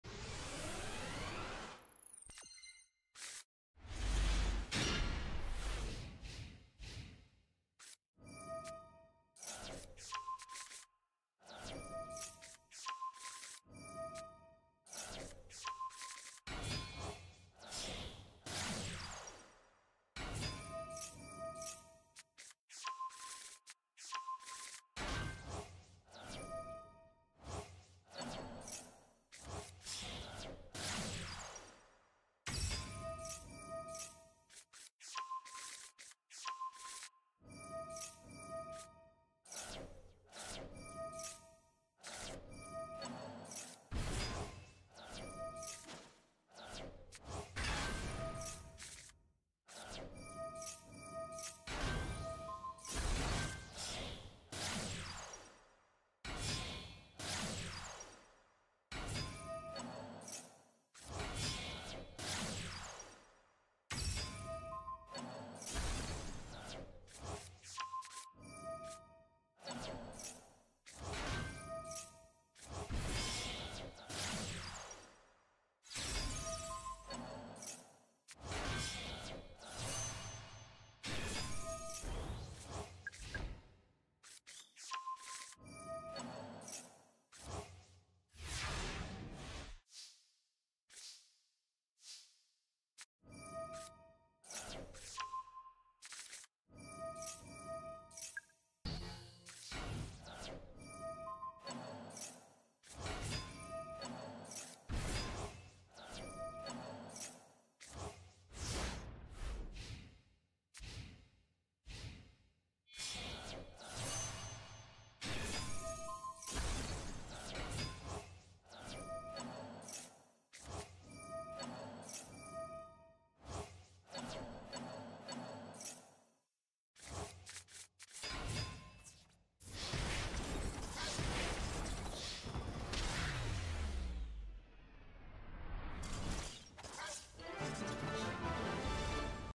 Master Cup 28/7 Top 4 Game 1
I won the top 8 match and managed to get into the top 4.
This match is a best of 3 and if I win, I go to the grand final.